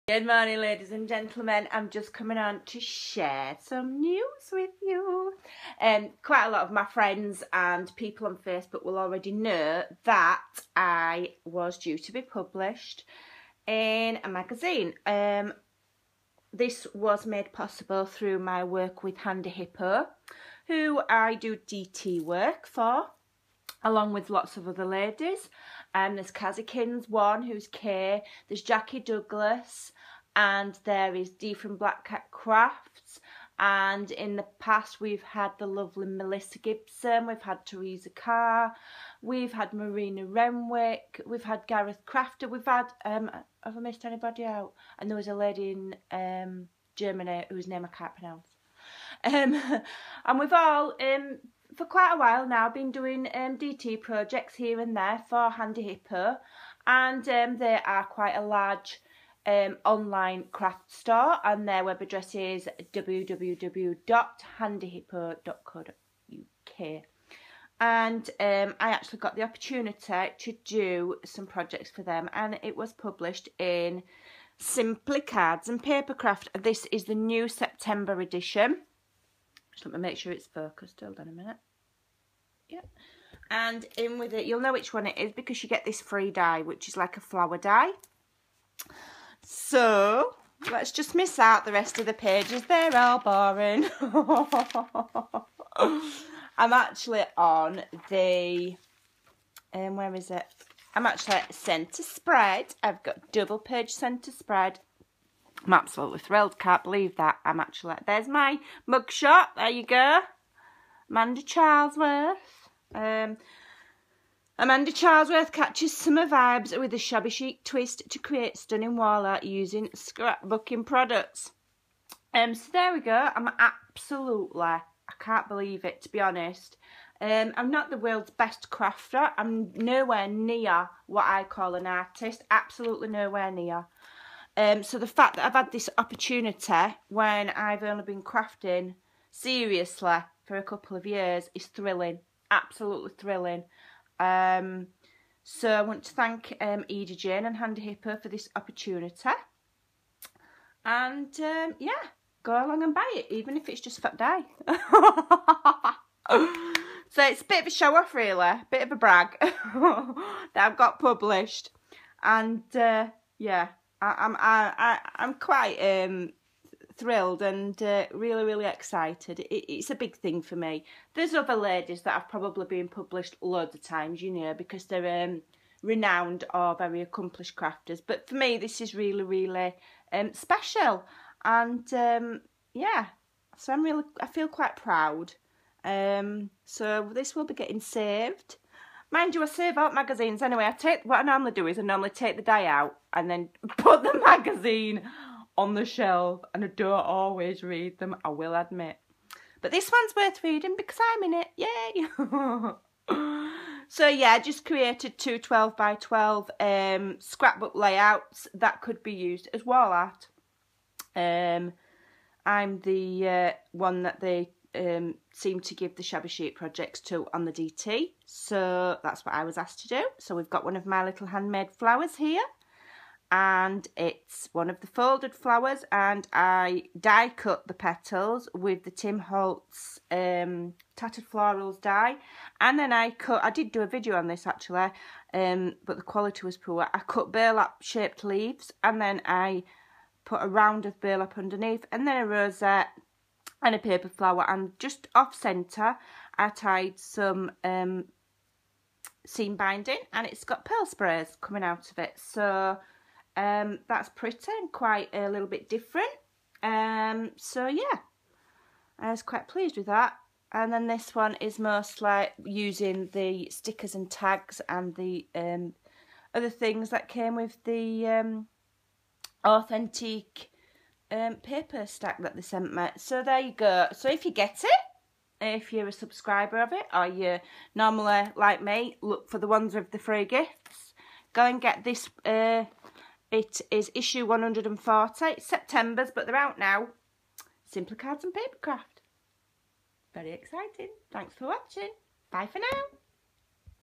Scrimpy gets published in Simply Cards and paper craft
Yay sharing my fantastic news I'm so excited to have had my work published